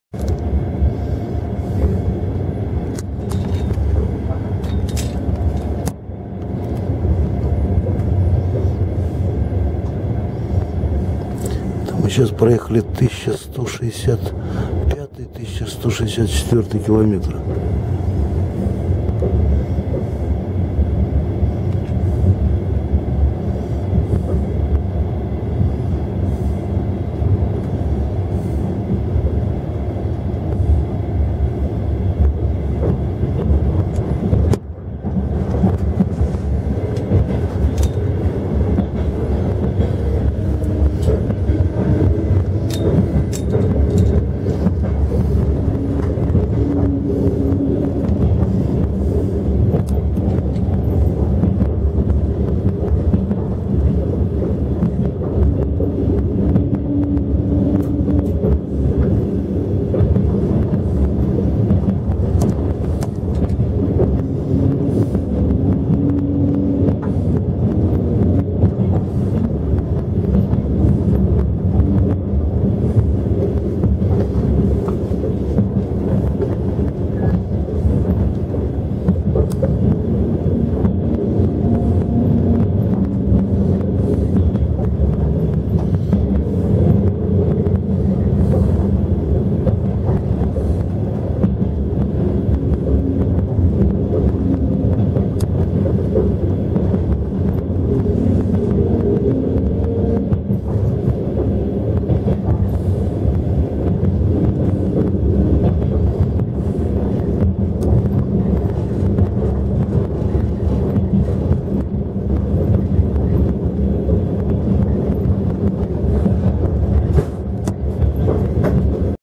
Поезд 30 Пейзажи Нестеровского района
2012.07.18 wed -0141.MOV Поезд № 30 Янтарь Калининград - Москва